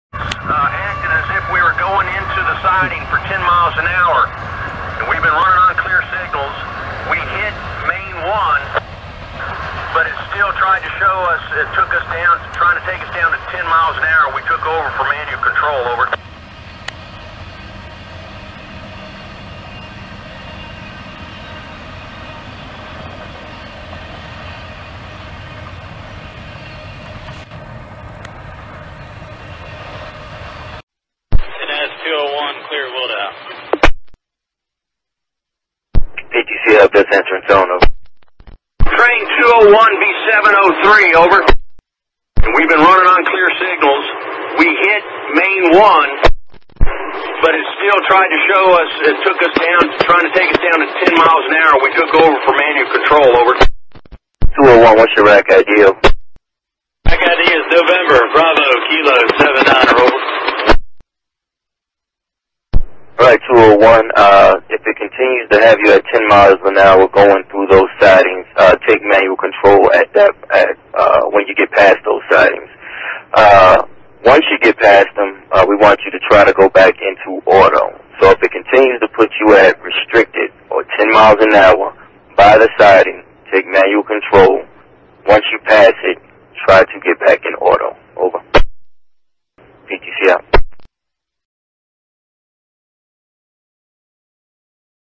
Hotshot 201 PTC Computer Problems At Wilda
Thursday October 4 2018 hotshot 201 came barreling by Wilda but from the conversation the Engineer was having with Atlanta Control indicated not all was well, in the Positive Train Control mode whenever the train came to a siding the computer wanted to slow the train to 10 mph! Atlanta Control gives a solution